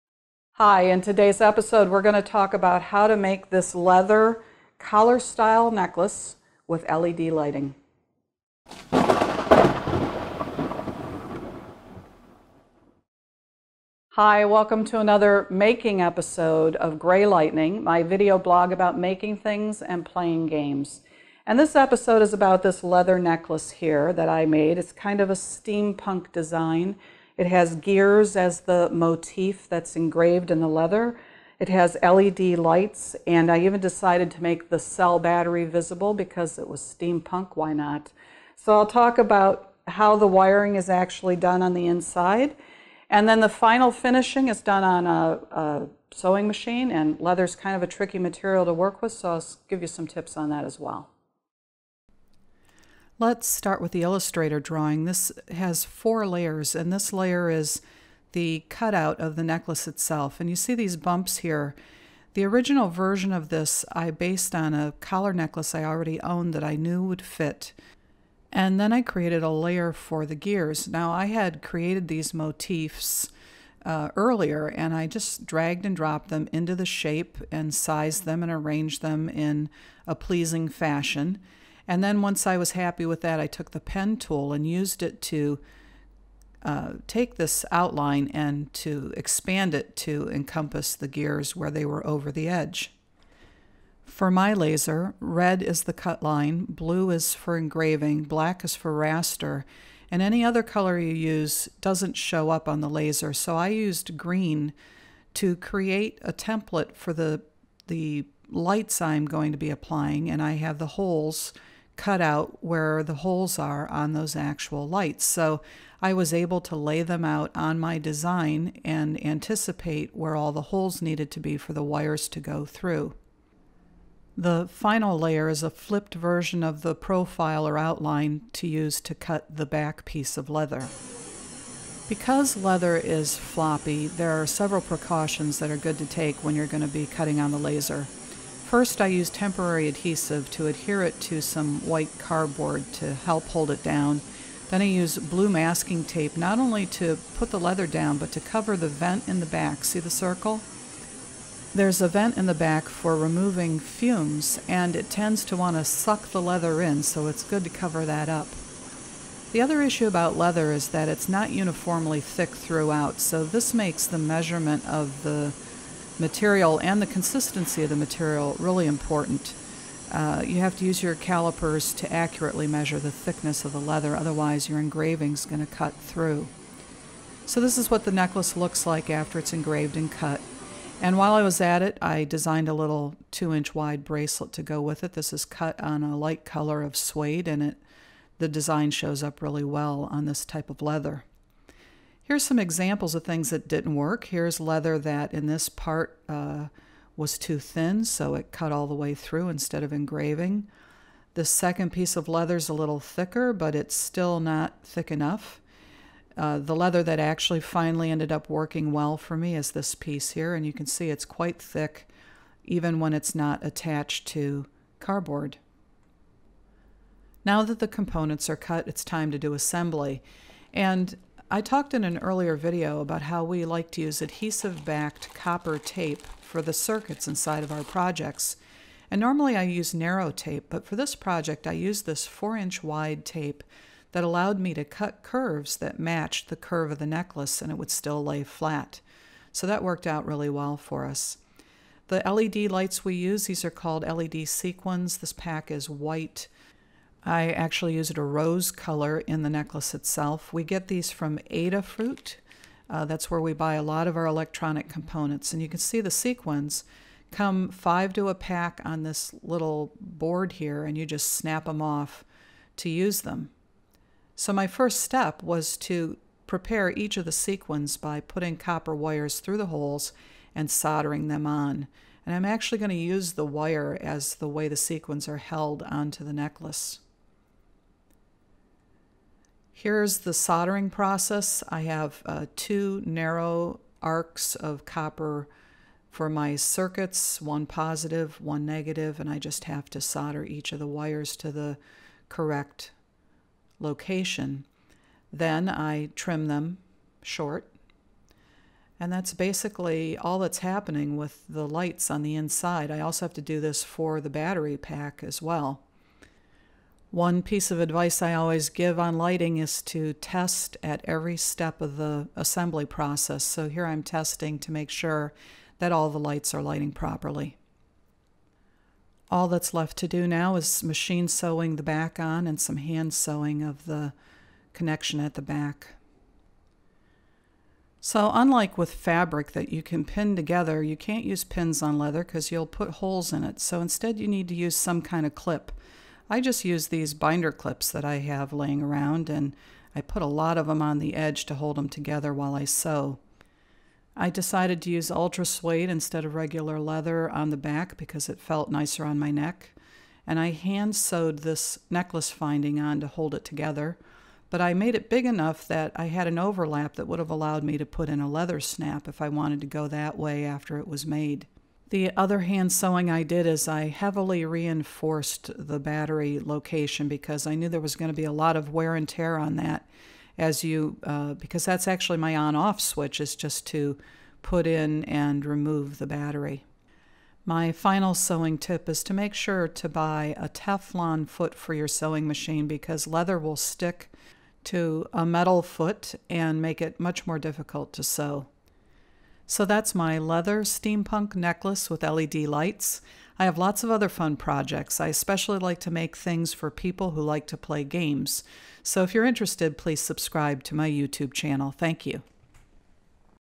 How to Make a Laser Cut Leather Steampunk Necklace with LED Lights
In this 'making' episode I show how to laser cut and assemble a steampunk collar necklace. This leather necklace has a gear motif and is lit with small rose colored LED lights. The design is done in Adobe Illustrator. I give some tips on how to cut leather on a laser cutter, and I also give some tips about sewing leather on a sewing machine.
I have created an Instructable for this project, which has step by step instructions and photos: